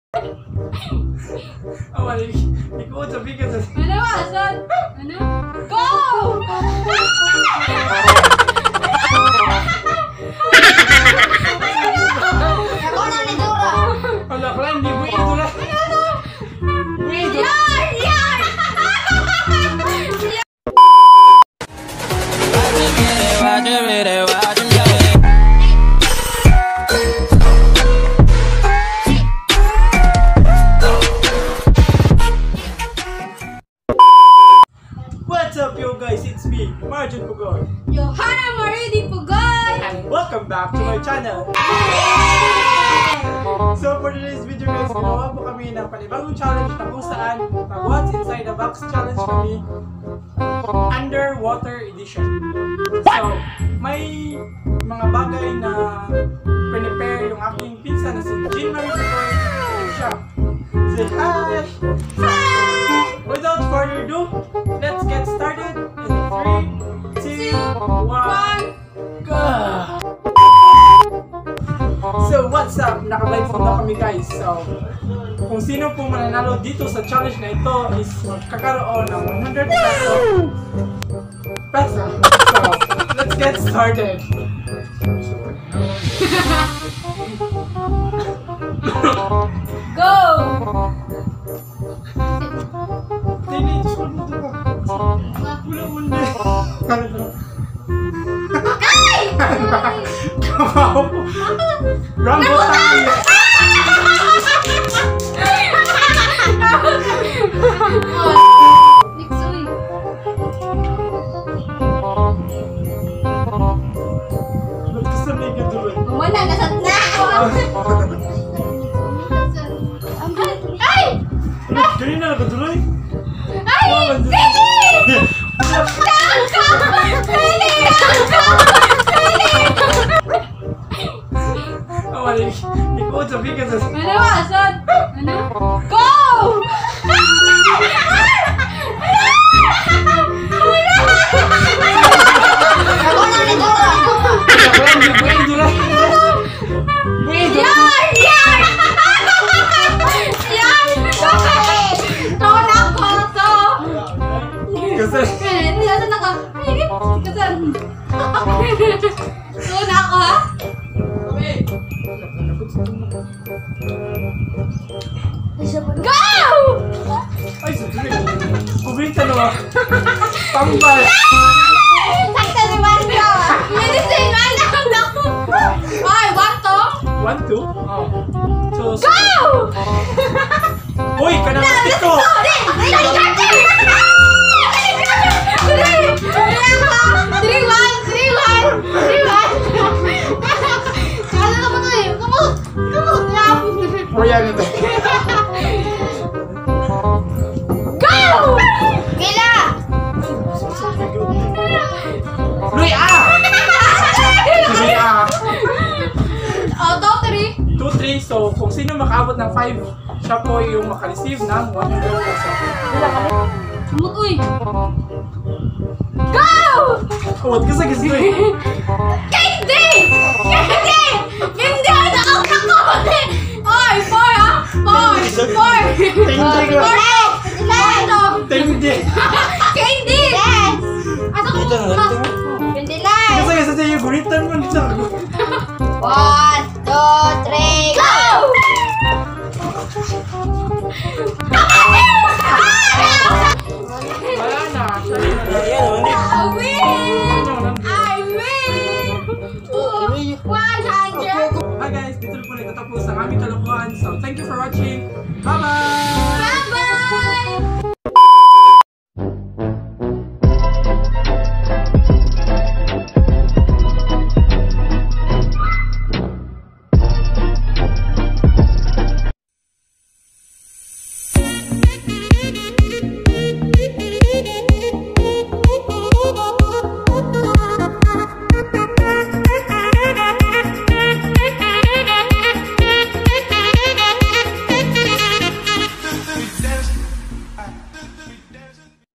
WHATS INSIDE THE BOX *WATER EDITION FT. JOHANNA POGOY//Marjon Pogoy
This video is all about whats inside the box with a twist   i lalagay ang mga bagay sa tupper ware na may tubig katuwaan lamang po ito .

FAQ:
Editor-KINEMASTER AND POWERDIRECTOR
Camera-VivoY81i